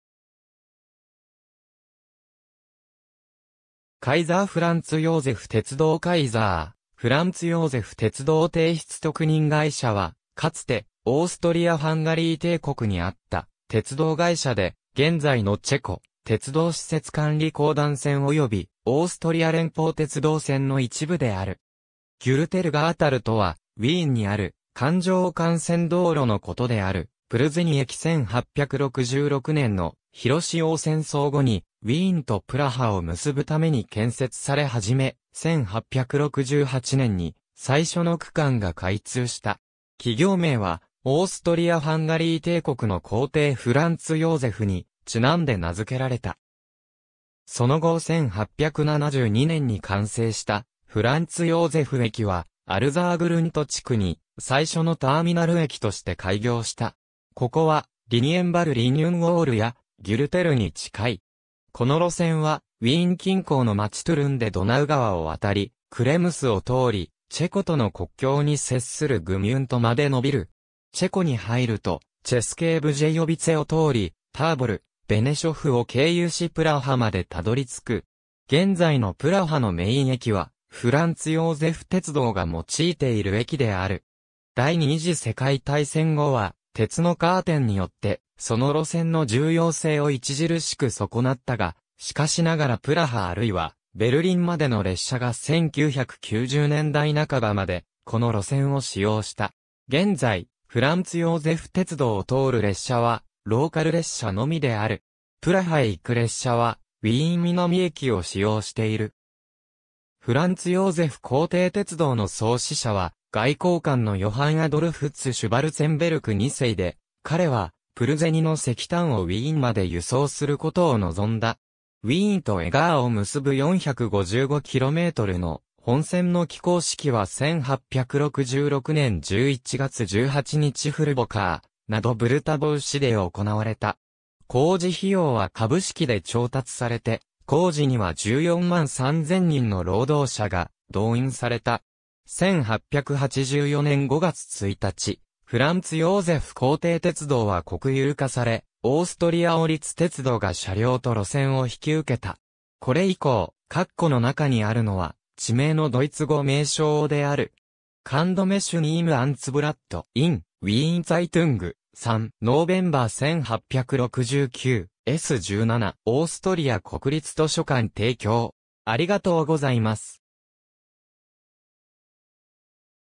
カイザー・フランツ・ヨーゼフ鉄道
カイザー・フランツ・ヨーゼフ鉄道 カイザー・フランツ・ヨーゼフ鉄道帝室特認会社（ドイツ語 : KFJB, k.k. privilegierte Kaiser-Franz-Josephs-Bahn）は、かつてオーストリア＝ハンガリー帝国にあった鉄道会社で、現在のチェコ・鉄道施設管理公団（SŽDC）線（チェコ国鉄線）およびオーストリア連邦鉄道（ÖBB）線の一部である。
ギュルテル Gürtel とはウィーンにある環状幹線道路のことである プルゼニ駅 1866年の普墺戦争後に、ウィーンとプラハを結ぶために建設され始め、1868年に最初の区間が開通した。
企業名はオーストリア・ハンガリー帝国の皇帝（カイザー）フランツ・ヨーゼフにちなんで名づけられた。
その後1872年に完成したフランツ・ヨーゼフ駅は、アルザーグルント（ウィーン9区）地区に最初のターミナル駅として開業した。
ここはリニエンヴァル Linienwall（ウィーンを囲む城壁）や、ギュルテルに近い。
この路線はウィーン近郊の町トゥルンでドナウ川を渡り、クレムスを通りチェコとの国境に接するグミュントまで伸びる。
チェコに入ると、チェスケー・ブジェヨヴィツェ（ベーミッシュ・ブデオヴァイス）を通り、ターボル、ベネショフ（ベネシャウ）を経由しプラハ（プラーク）まで辿りつく。
現在のプラハのメイン駅は、フランツ・ヨーゼフ鉄道が用いている駅である。
第二次世界大戦後は鉄のカーテンによって、その路線の重要性を著しく損なったが、しかしながらプラハあるいはベルリンまでの列車が1990年代半ばまでこの路線を使用した。
現在、フランツ・ヨーゼフ鉄道を通る列車はローカル列車のみである。
プラハへ行く列車はウィーン南駅を使用している。
フランツ・ヨーゼフ皇帝鉄道の創始者は外交官のヨハンアドルフ・ツ・シュヴァルツェンベルク二世 (Johann Adolf II. zu Schwarzenberg) で、彼はプルゼニの石炭をウィーンまで輸送することを望んだ。
ウィーンとエガー (現在ヘプ) を結ぶ455 kmの本線の起工式は1866年11月18日フルボかー・ナド・ヴルタヴォウ市で行われた。
工事費用は株式で調達されて、工事には14万3000人の労働者が動員された。
1884年5月1日フランツ・ヨーゼフ皇帝鉄道は国有化され、オーストリア王立鉄道が車両と路線を引き受けた。
^ これ以降、括弧の中にあるのは地名のドイツ語名称である。
^ Kundmachung im Amtsblatt. In: Wiener Zeitung, 3. November 1869, S. 17 (Online bei ANNO). オーストリア国立図書館提供